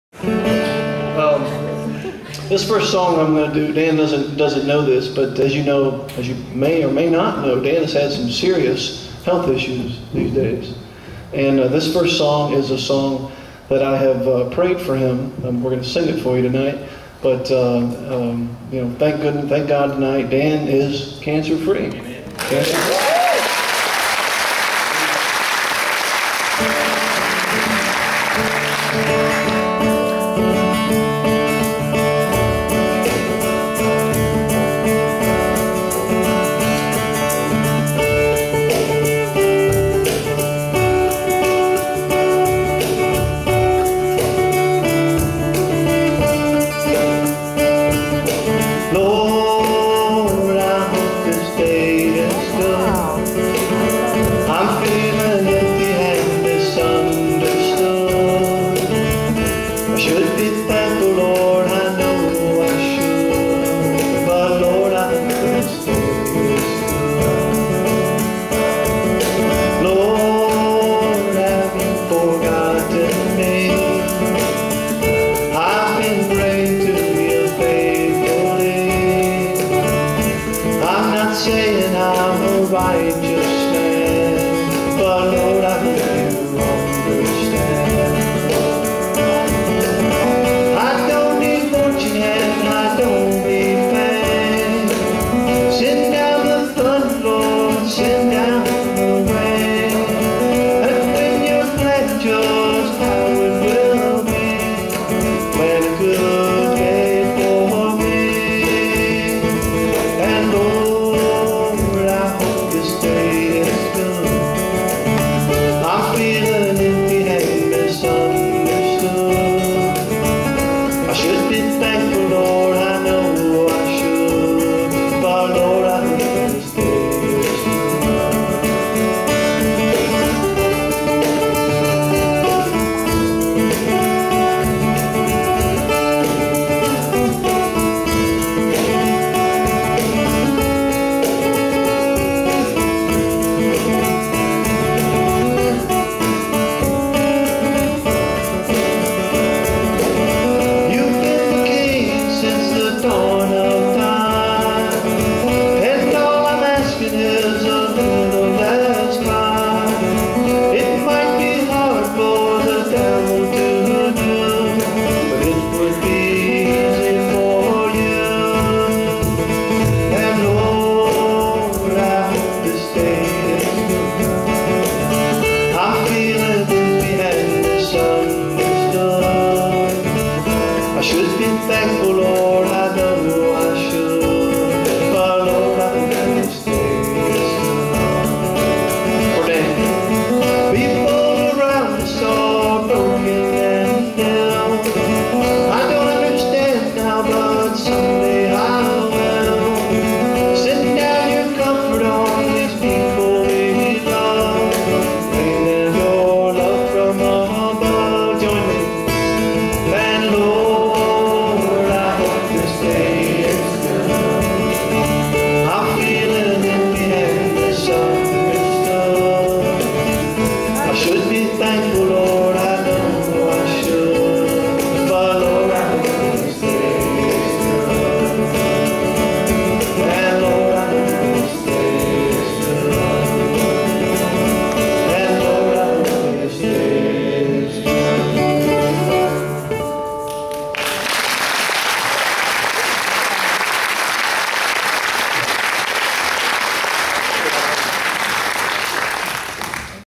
KMW Performance
Performed by Bruce W. Ford, Dan Landrum and Emma Ford at Kentucky Music Week (KMW), Bardstown Kentucky - 20 June, 2010